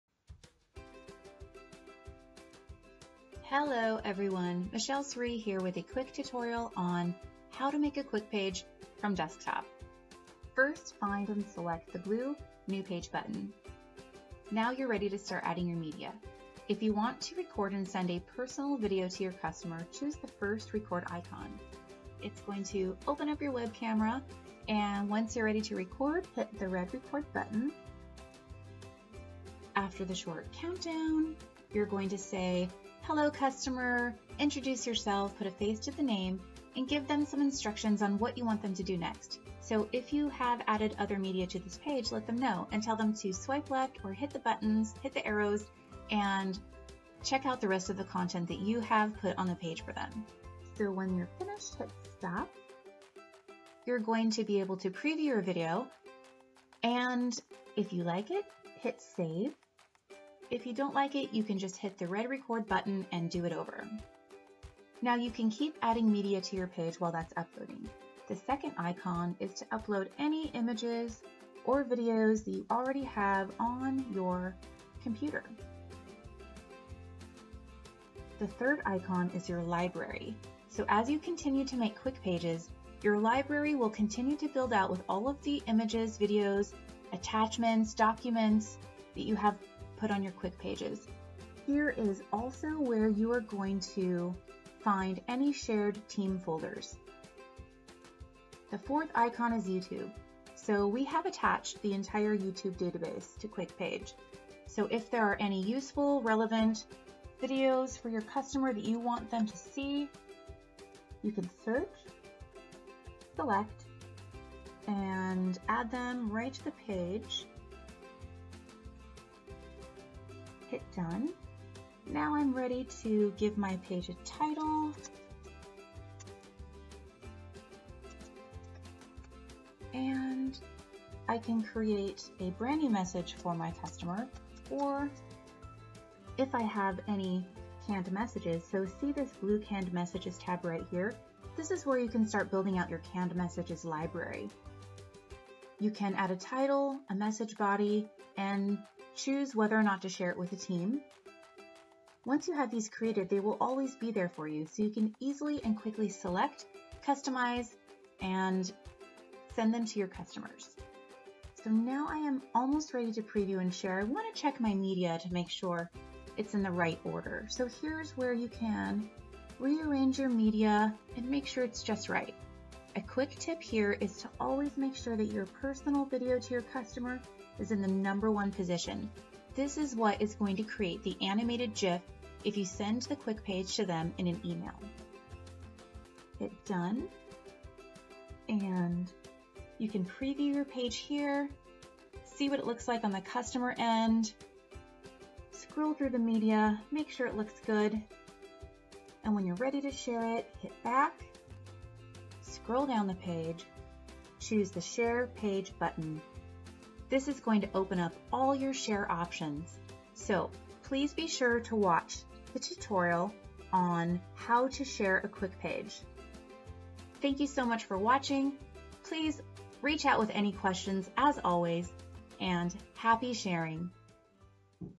How to Make a Quickpage Video Email Desktop Tutorial
A quick video tutorial on how to make a Quickpage video email from your desktop computer.